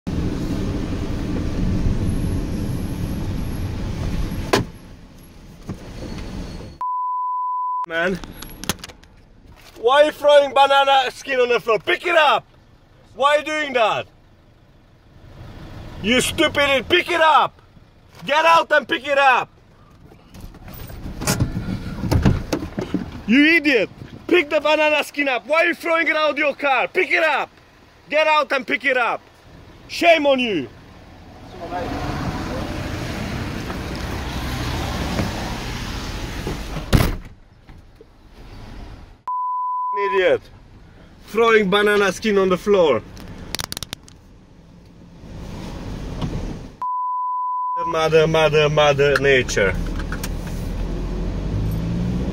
Man Argues With Banana Thrower
A man was told to pick up his litter after throwing his banana skin out of his car window.

Istvan Otvos, 34, was driving on May 17th in Hemel Hempstead when he noticed the driver in front throw litter onto the road.

The van driver pulled up next to the litterer in a vicious rage, making him get out of his car and pick up the banana skin.